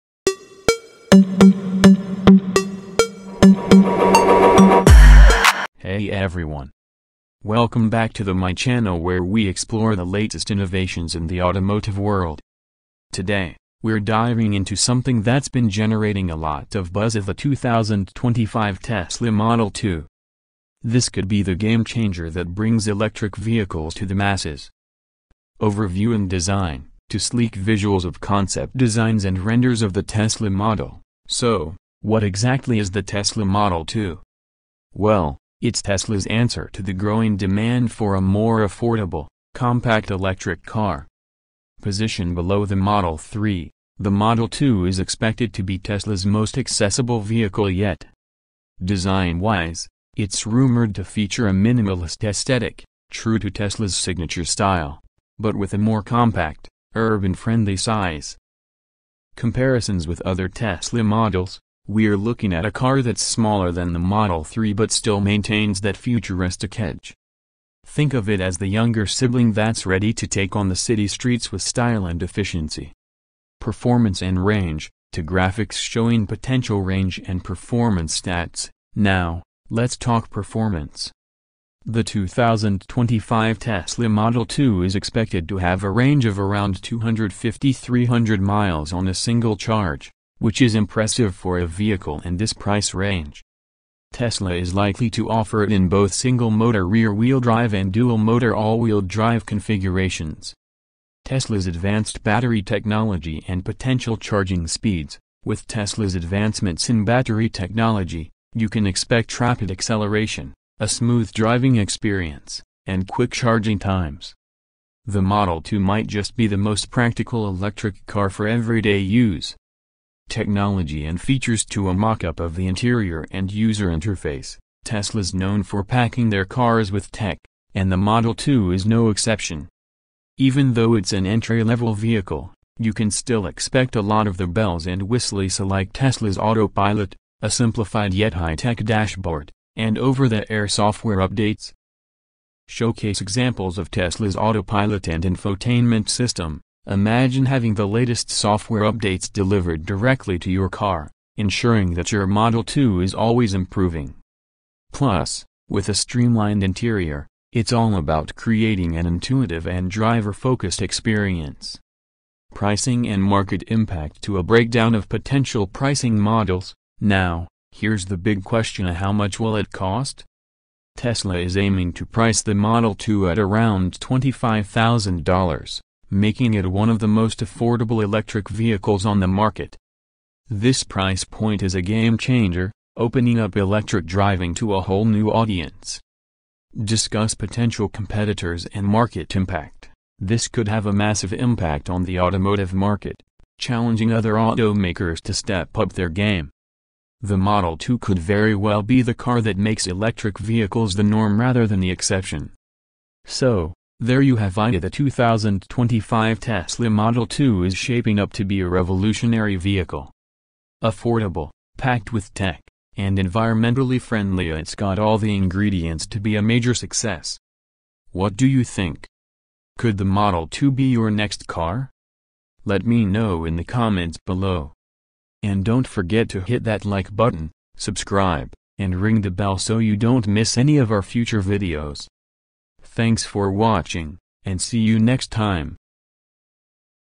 2025 Tesla Model 2: Specs, Release Date, and What to Expect"
Get ready to explore Tesla's newest innovation—the 2025 Tesla Model 2! Designed to make electric driving more accessible than ever, the Model 2 promises impressive range, cutting-edge technology, and an affordable price tag. In this video, we dive into the key features, potential release date, and what this game-changing EV could mean for the future of transportation. Is this the budget-friendly Tesla we've all been waiting for? Watch to find out!